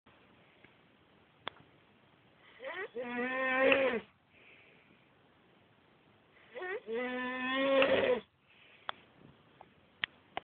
me snoring
this is genuinly a video of me snoring unfortunatly sarah didn't know how to put the light on my phone so you can't actually see anything worth a listen though